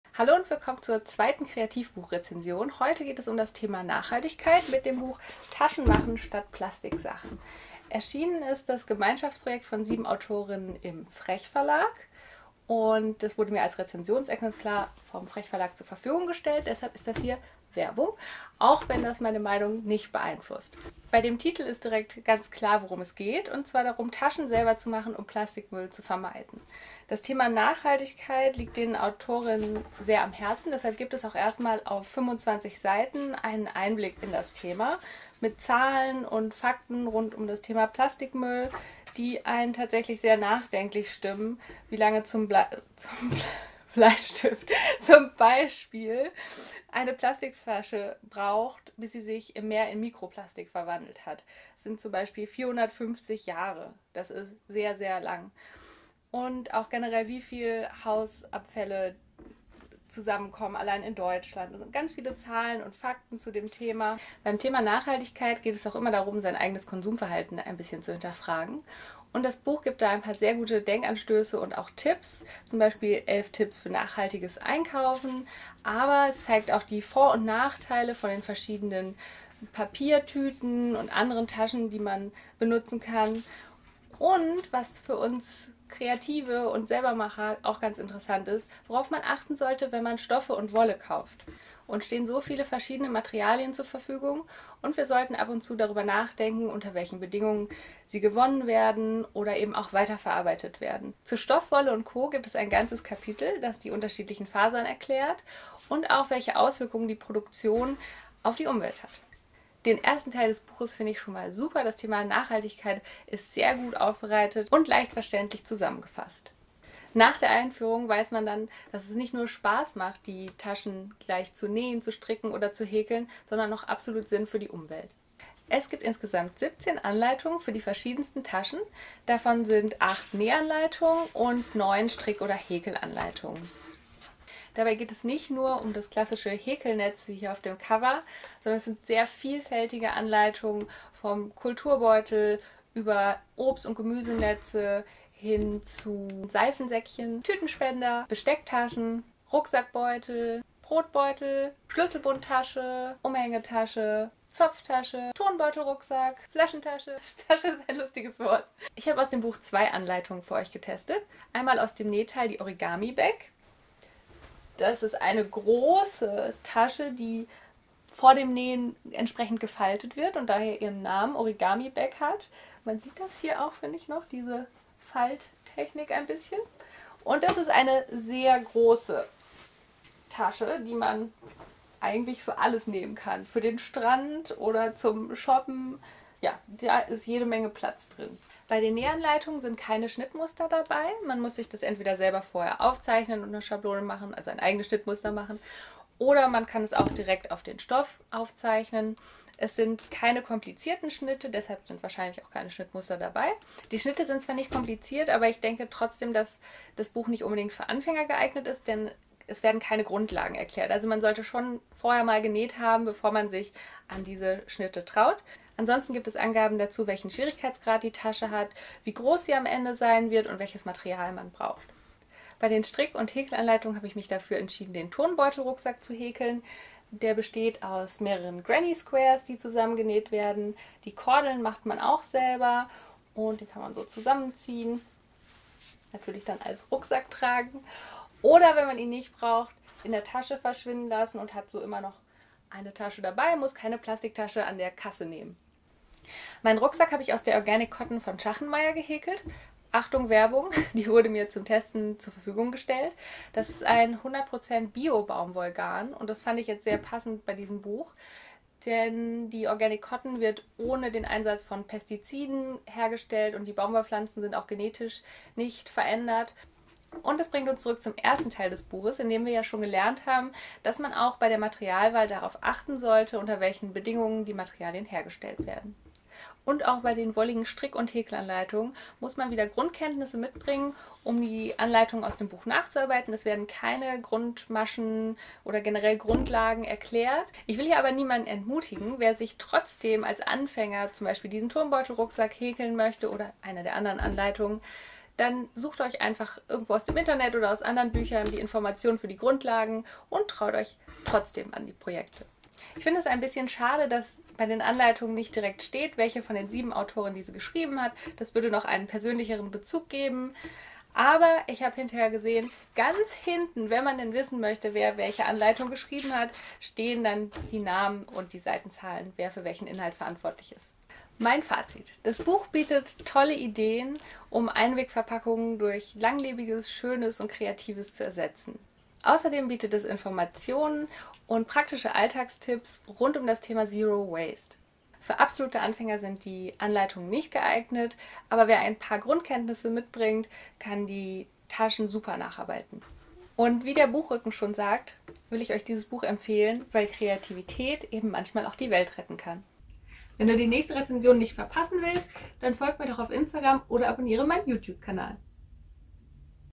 Sapri Design - Buch-Rezension - Taschen machen statt Plastik Sachen
Wenn ihr jetzt Lust bekommen habt die Taschen selber zu nähen, zu stricken oder zu häkeln, hier noch mal alle Infos zum Buch auf einen Blick:
"Taschen machen statt Plastik Sachen"
von Manuela Gaßner, Jasmin Konrad, Katharina Spreng, Eva Scharnowski, Frederike Mathhäus, Ramona Zirk, Manuela Seitter
erschienen im Frechverlag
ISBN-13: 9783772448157
Hardcover / 120 Seiten / 19,2 x 1,7 x 25,1 cm
17,-€